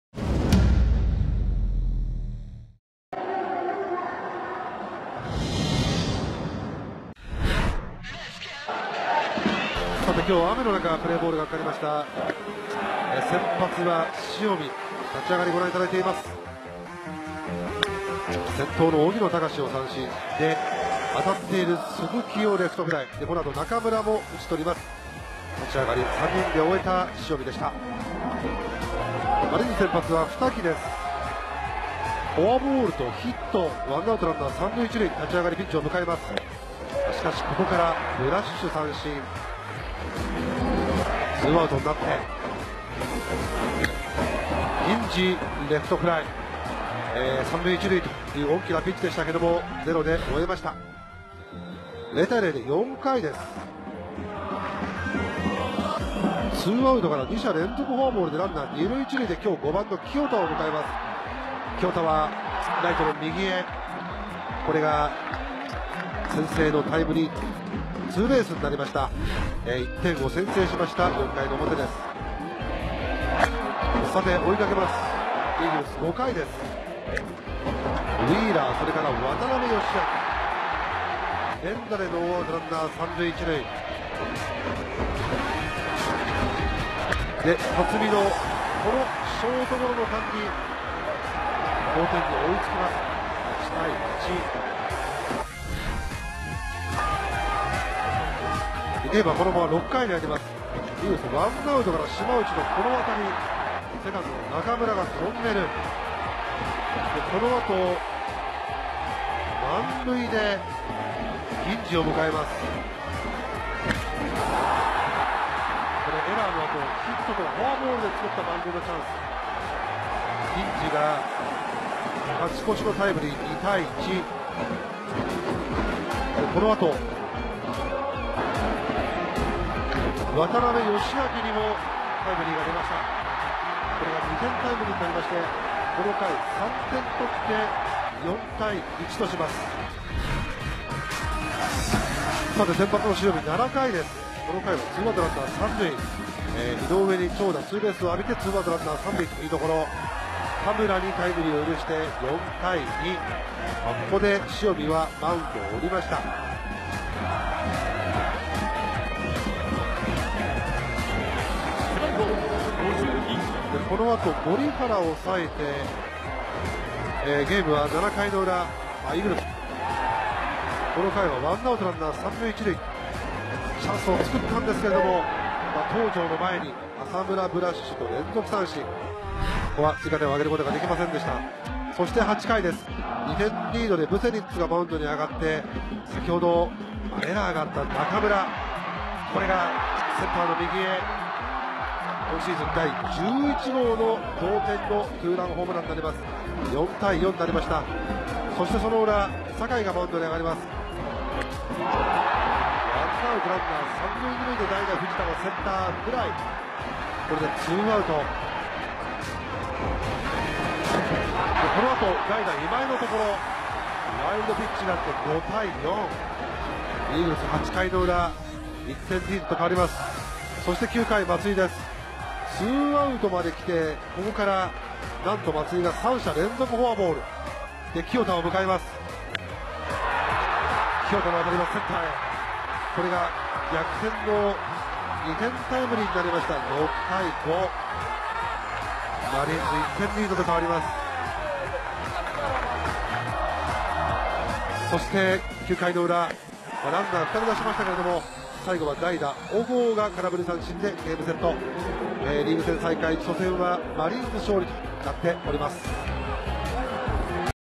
2019年6月28日 東北楽天対千葉ロッテ 試合ダイジェスト
イーグルス対マリーンズ(楽天生命パーク宮城)の試合ダイジェスト動画。 2019/06/28 東北楽天ゴールデンイーグルス 対 千葉ロッテマリーンズ